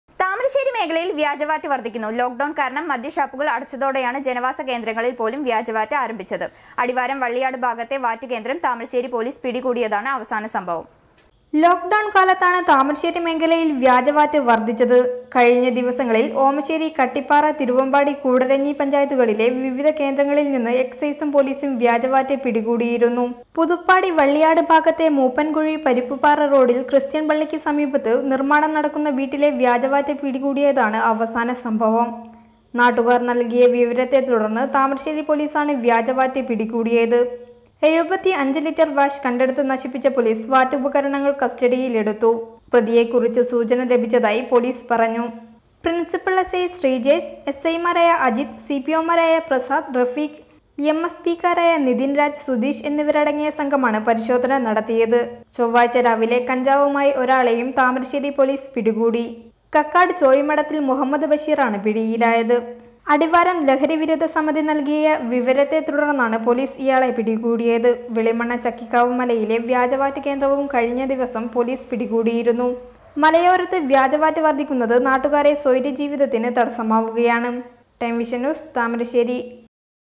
താമരശ്ശേരി മേഖലയില്‍ വ്യാജ വാറ്റ് വര്‍ധിക്കുന്നു Fake VAT on the rise in Thamarassery region
ലോക് ഡൗണ്‍ കാരണം മദ്യശാപ്പുകള്‍ അടച്ചതോടെയാണ് ജനവാസ കേന്ദ്രങ്ങളില്‍ പോലും വ്യാജവാറ്റ് ആരംഭിച്ചത്. അടിവാരം വള്ളിയാട് ഭാഗത്തെ വാറ്റ് കേന്ദ്രം താമരശ്ശേരി പോലീസ് പിടികൂടിയതാണ് അവസാന സംഭവം. Fake VAT on the rise in Thamarassery region